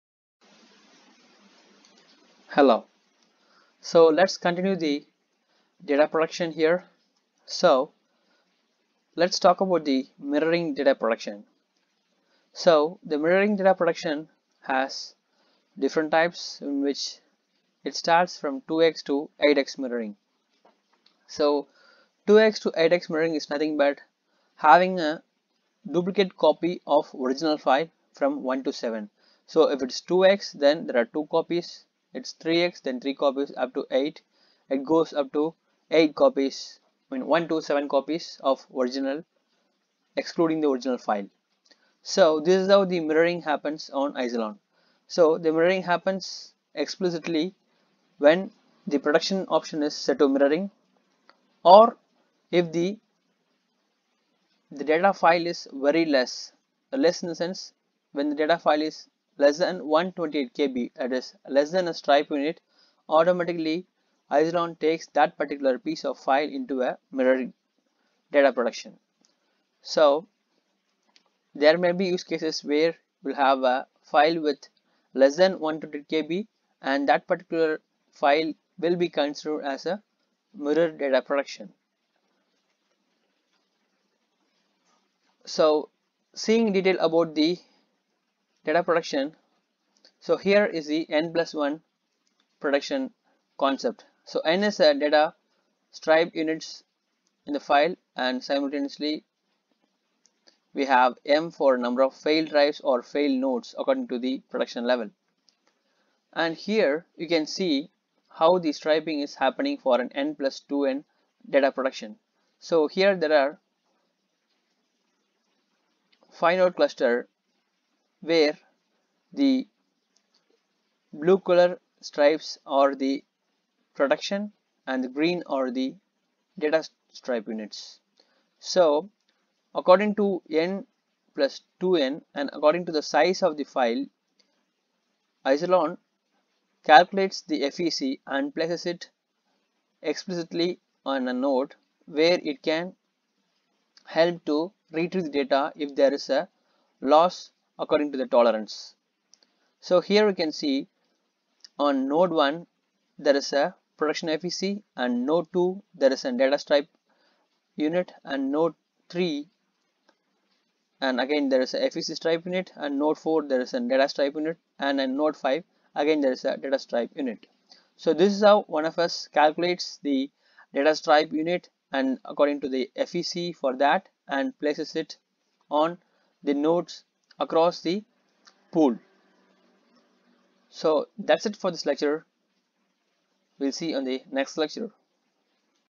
How is FEC Calculated in Isilon/ Power scale | Isilon Administration and Management
As the system writes the data, it also protects the data with parity bits called Forward Error Correction (FEC). The OneFS operating system spreads the data across numerous drives in multiple nodes so that if part of the data goes missing, the missing data can be recalculated and restored. This involves complex mathematics, but to illustrate the concept, we’ll use a basic example using three colored boxes with some contents. Let’s assume “5” and “3” be the contents of the blue and yellow boxes, respectively, on the first row. The sum of these two numbers is “8”, which we are calling a parity bit, and is the contents of the green box. For here on, once we know the contents of these three colored boxes we can successfully tell what value inside of a missing box on any of the subsequent rows of boxes, can’t we? That is, on the third row, knowing the contents of the blue and green boxes as “5” and “8” we can conclude the value of the missing yellow box to be “3”. In general, the basic idea of the parity bit is a value that OneFS sets to show what total should result when the binary data is added together.